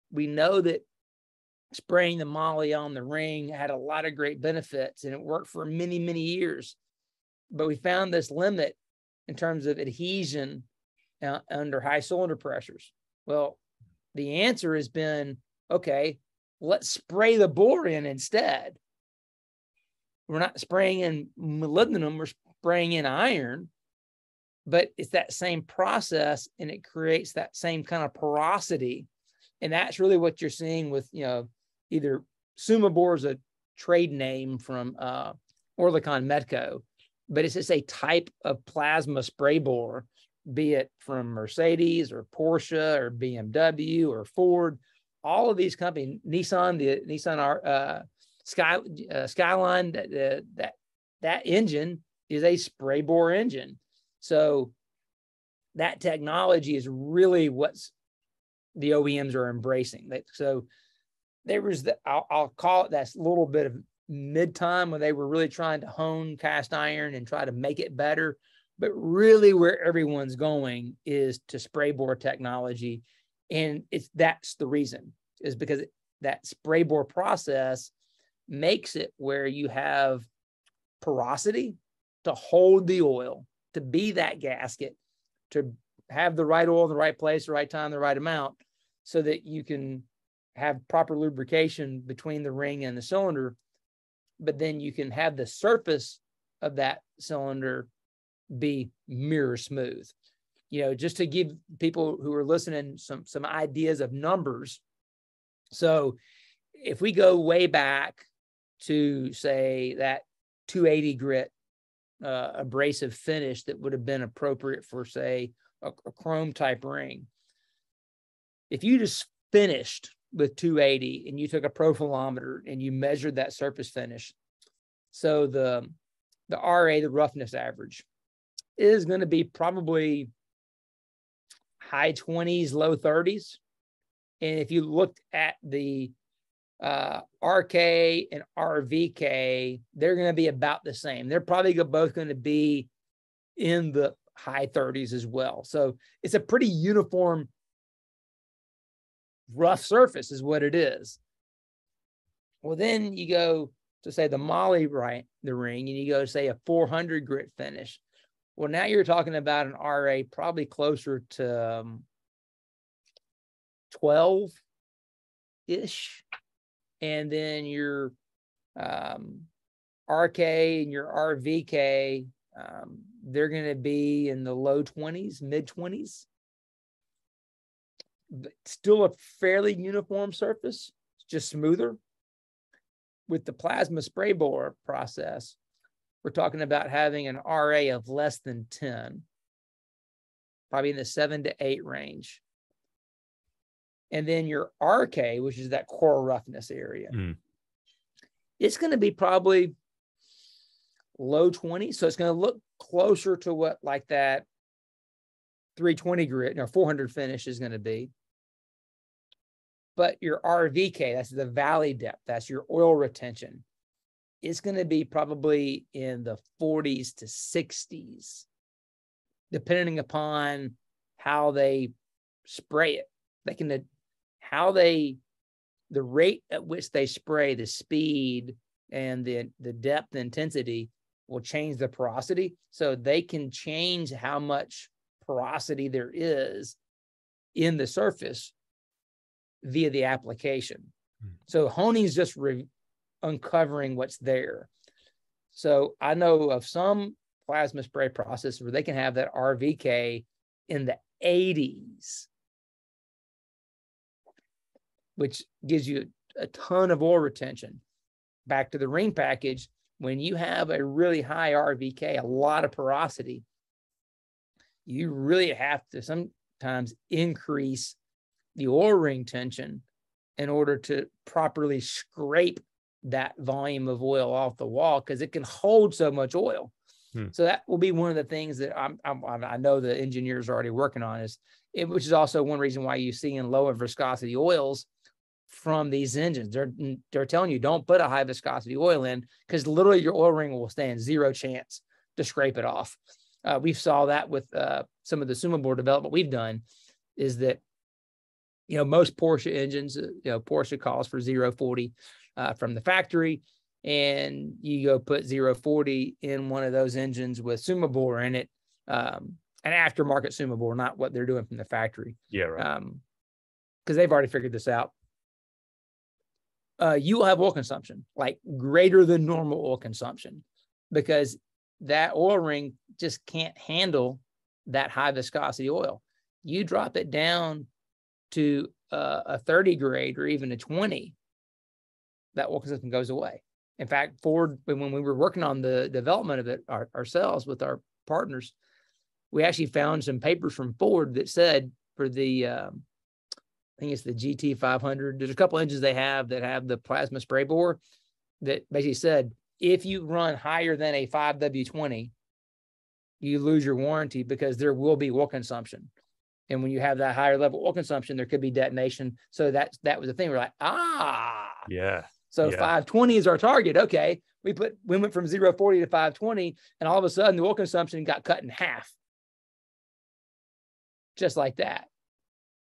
How new cylinder technologies are enabling lower viscosity oils
Most people don't like the new trend toward lower viscosity engine oils. But in some cases, there are actually good reason to not increase the oil viscosity beyond the spec that has been determined by the OEM. Lake Speed Jr help us understand some of the new cylinder finishing technologies that are helping to enable low viscosity oils.